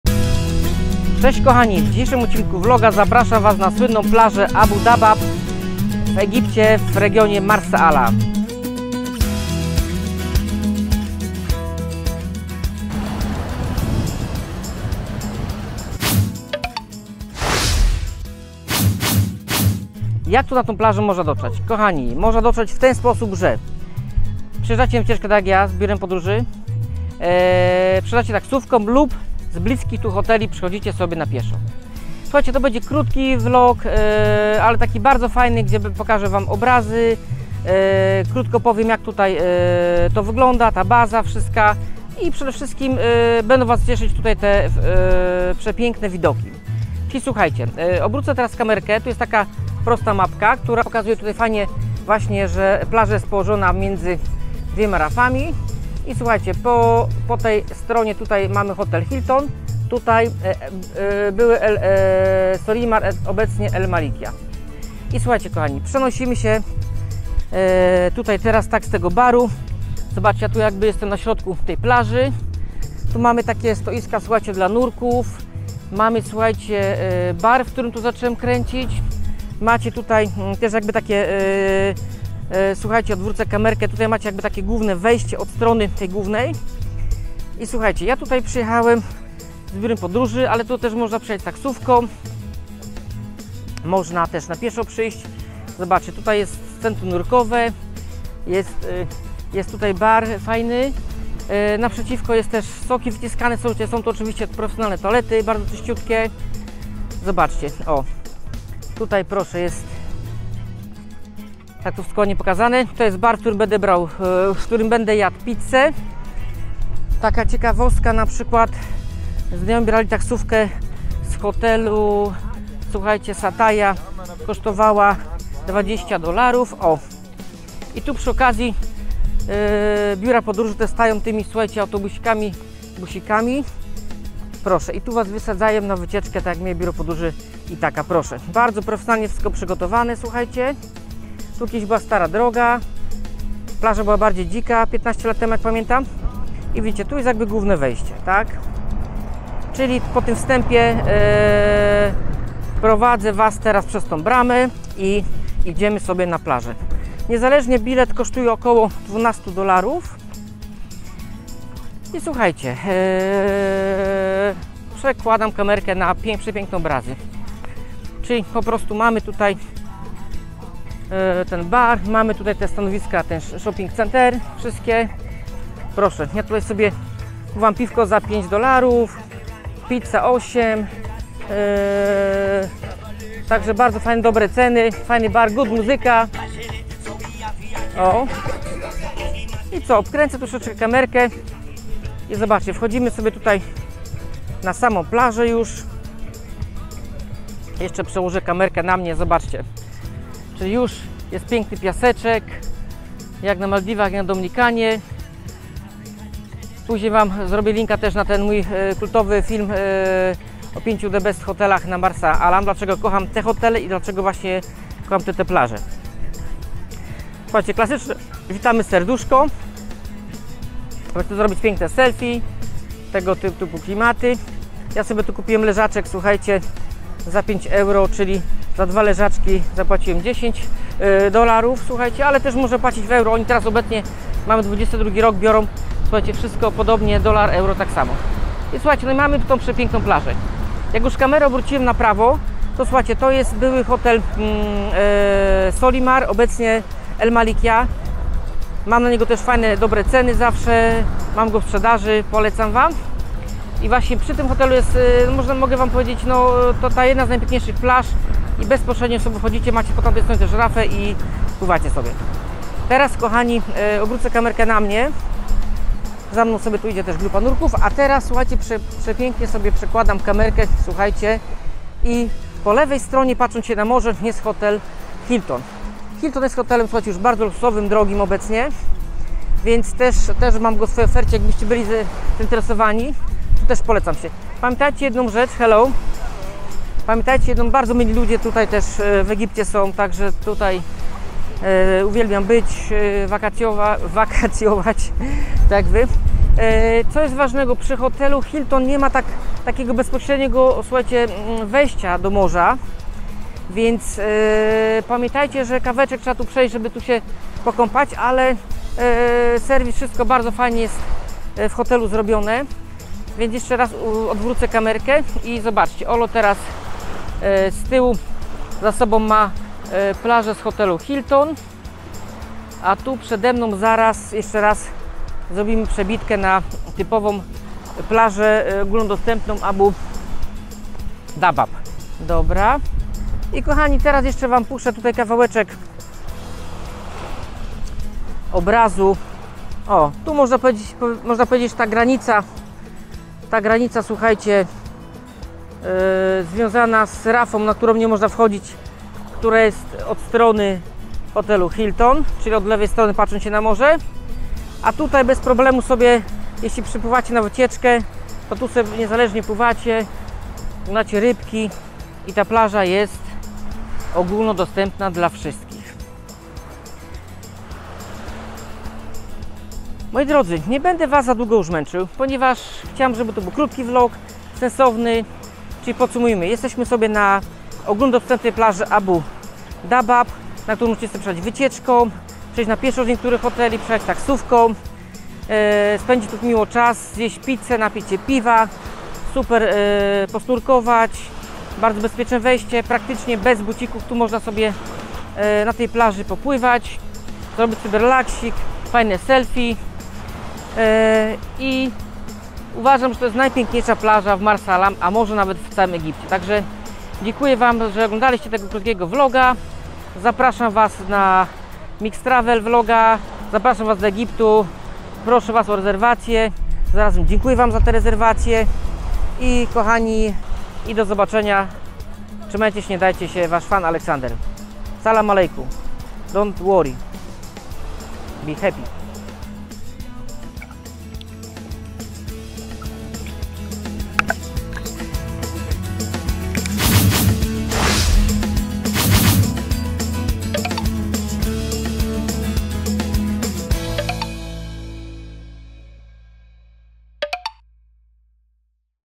Abu Dabbab Beach - Egipt | Mixtravel vlog odcinek 93
Plaża Abu Dabbab - Mara Alam - Egipt to kolejny odcinek vloga. Zobaczcie tą plażę razem ze mną.